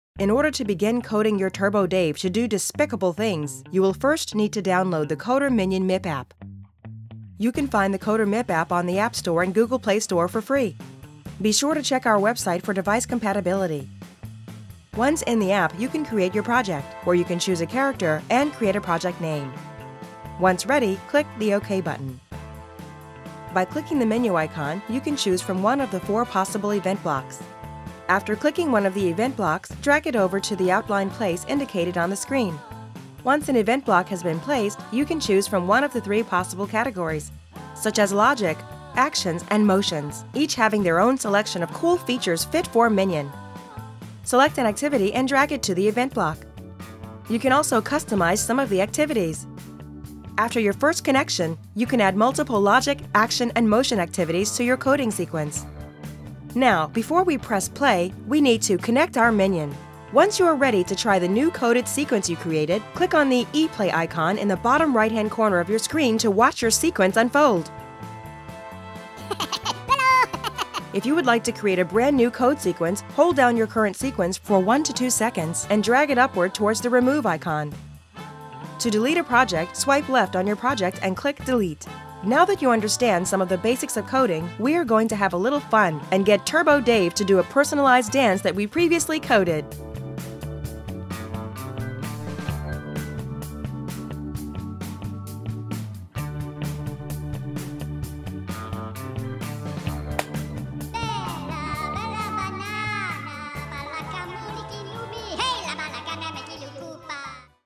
WowWee Minion MiP Turbo Dave Robot - How to Program your Robot
Minion MiP Tutorial #4: Learn to program Turbo Dave's responses to various inputs and create fun programming projects with the easy-to-use block-based programming app.